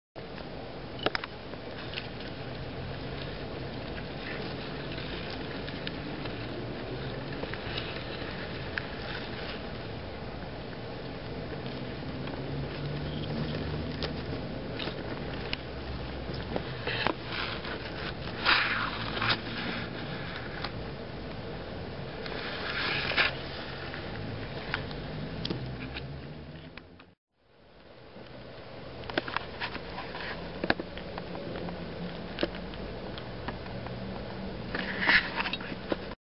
Wild mushrooms
Wild mushrooms sprang up overnight in my garden. September 2010, London, England. Original footage is better quality than it looks on YouTube.